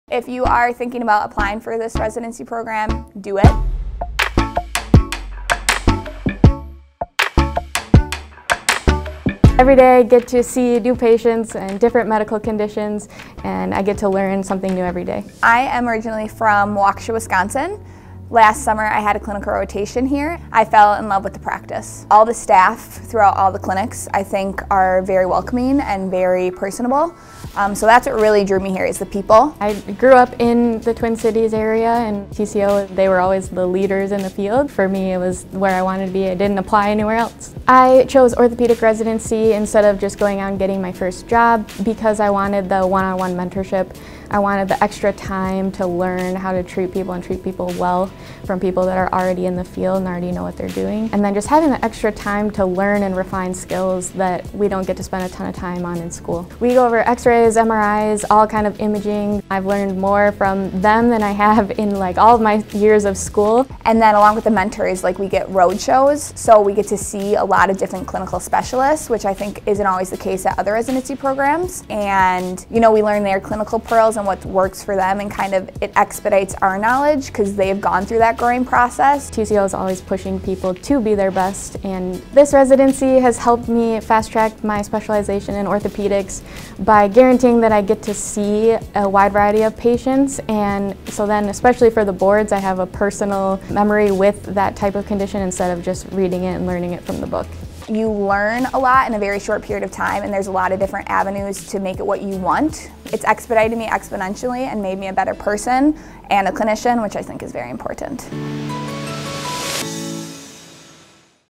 Orthopedic Physical Therapy Residency at Twin Cities Orthopedics
The Twin Cities Orthopedics (TCO) Orthopedic Physical Therapy Residency is a 54-week program beginning the 3rd week in July and concluding July 31 the following year. The residency is conducted at prominent locations within the Twin Cities Orthopedics organization.

We are dedicated to developing physical therapists who will become leaders in orthopedic rehabilitation. Our mission is to train physical therapists of diverse experiences and talents to become ambassadors of change, enabling our graduates to transform the field of physical therapy by providing compassionate, science-based physical therapy for patients of all needs and backgrounds. Our clinical training is built upon a challenging curriculum, dedicated mentorship, and institutional support to provide individualized experiences to facilitate the professional goals of each physical therapy resident.